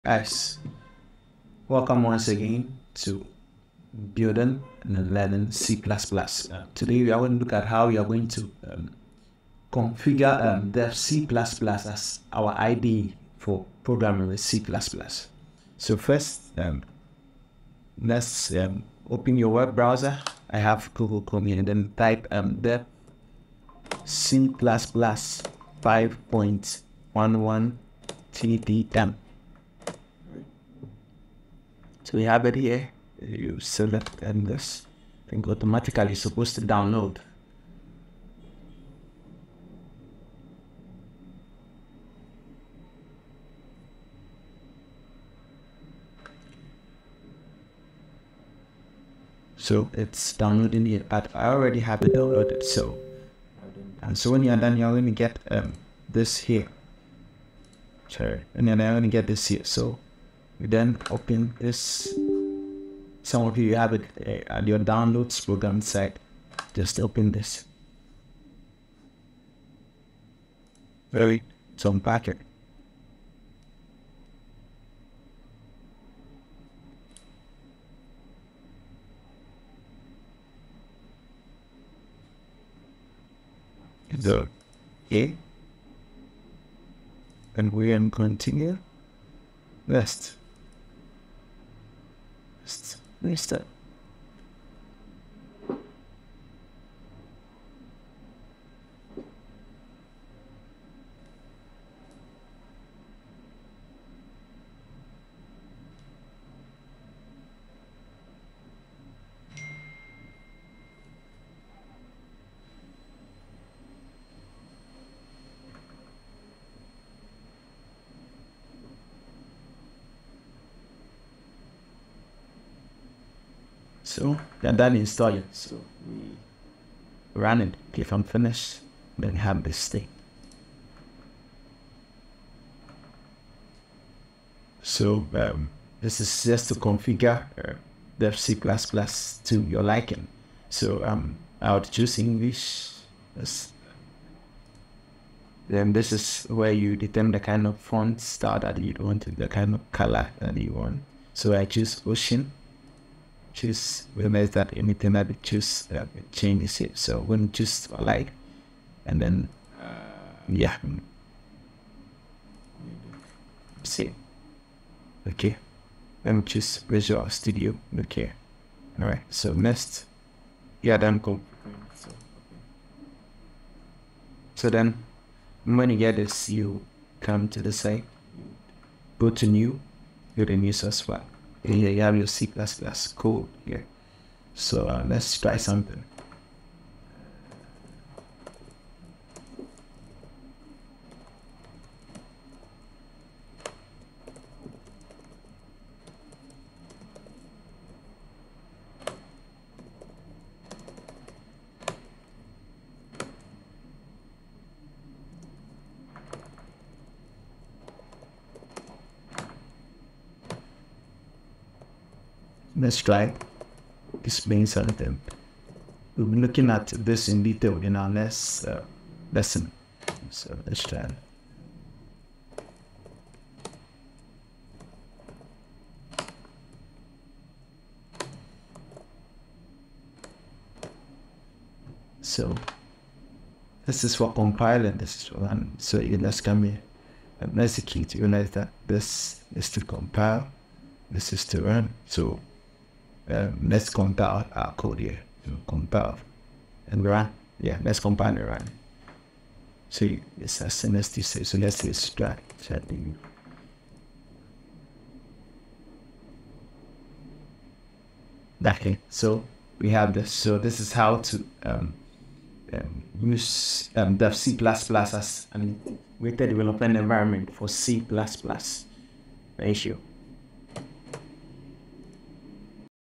Learn C++ Programming using Dev C++ as IDE : Beginner's Step by Step guide
"In this video, we'll walk you through the process of installing and using Dev C++ as an Integrated Development Environment (IDE) for C++ programming. Whether you're new to coding or looking to switch to a new IDE, this tutorial will help you set up Dev C++ and start writing and compiling your first C++ programs. Watch as we cover the basics and provide tips to enhance your coding experience. DevC++ C++Programming CodingTutorials"